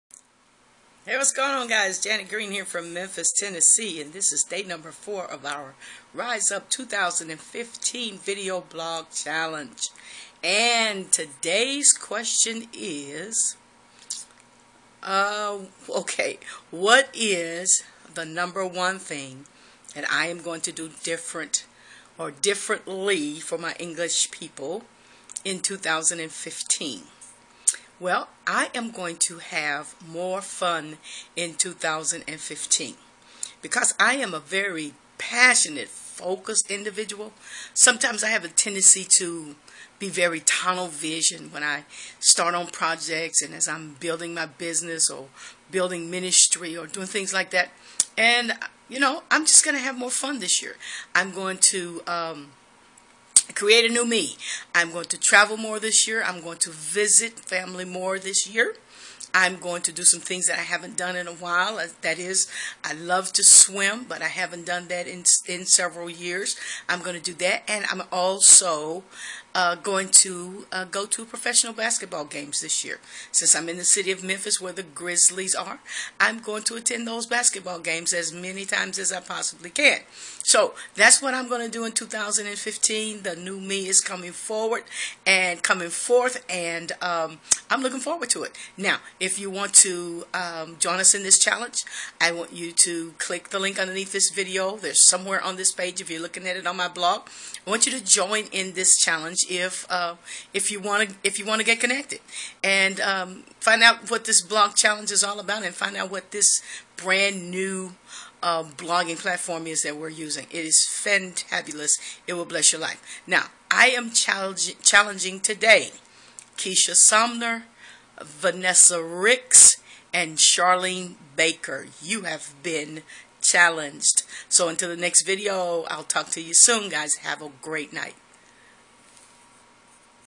Riseup2015 Vlog Challenge Day 4
Day 4 of our riseup2015 Vlog Challenge. The question for today's vlog challenge is What is the #1 thing that I am going to do differently in 2015.

Find out the answer. Then join us in this Rise Up 2015 Vlog challenge by clicking on one of the banners on my blog.